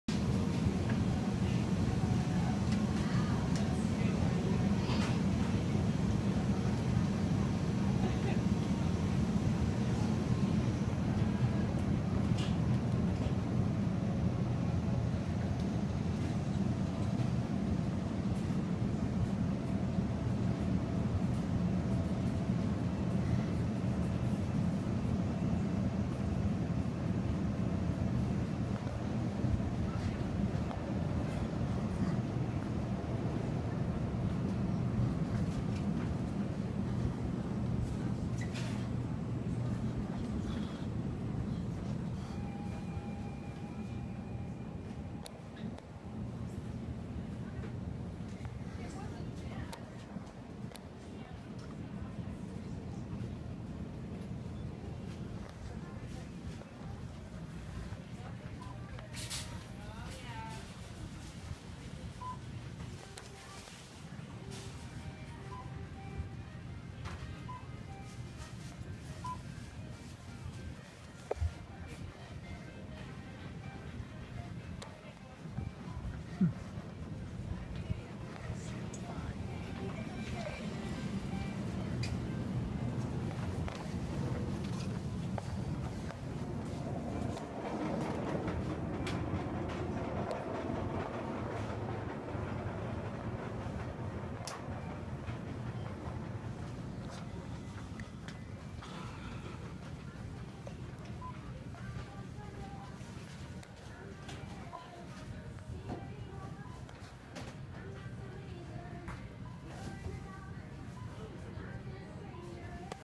6 NuTone Pro-Line II industrial ceiling fans at Crosby's (Greatest Hits Remake)
Second generation NuTone Pro-Line industrial ceiling fans, two are off, last time only one was off, don't know if the other one broke down since I was last here, enjoy